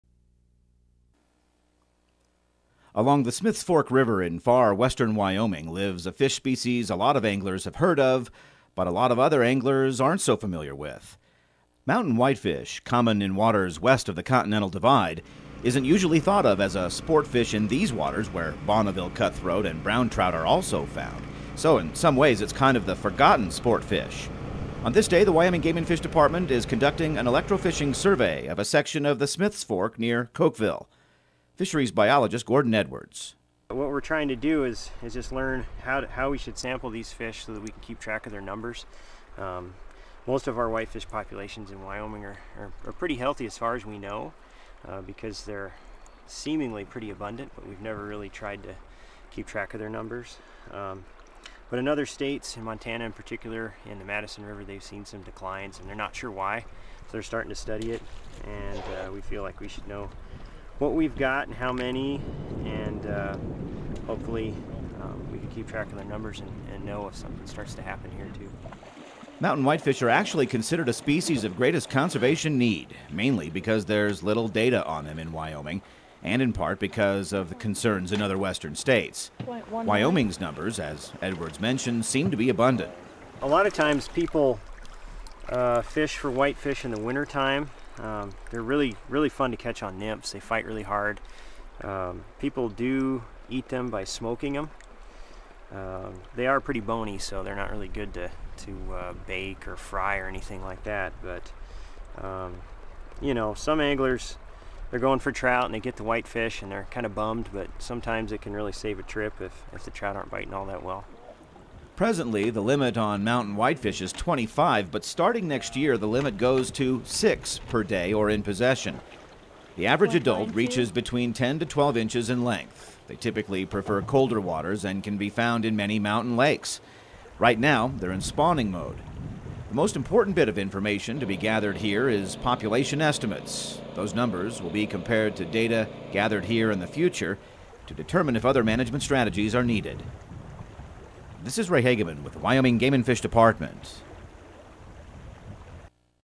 Mountain Whitefish
Sometimes called the "forgotten sportfish," the mountain whitefish can be fun to catch. See how WGFD biologists are studying whitefish populations in Wyoming and what they are learning.